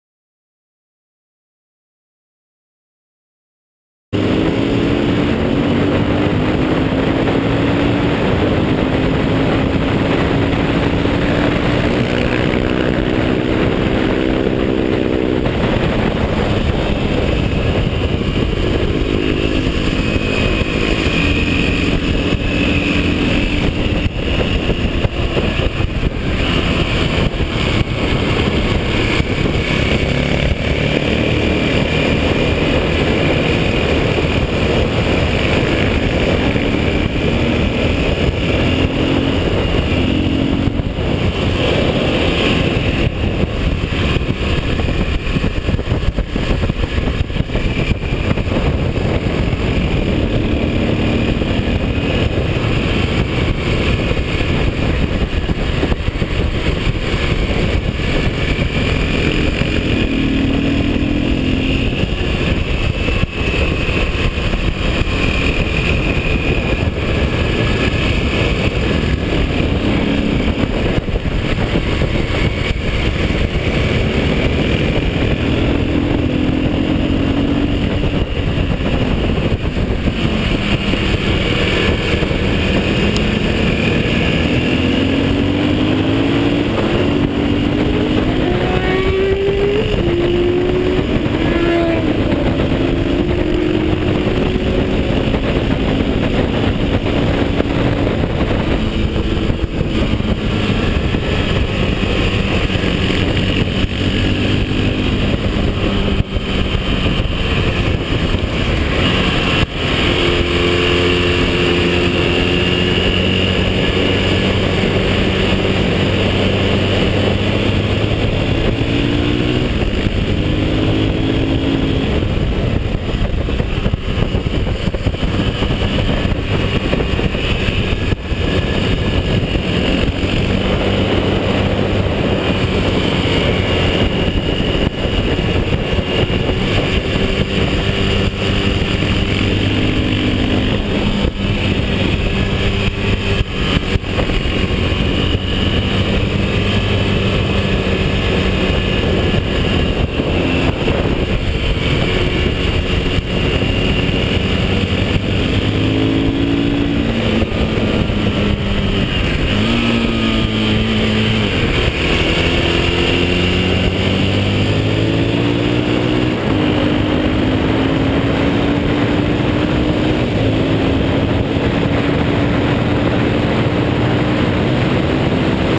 Trackday at Kinnekulle ring 2012 - Erik Larsson
Erik's first day at Kinnekulle Ring.
Laptime: about 1m21s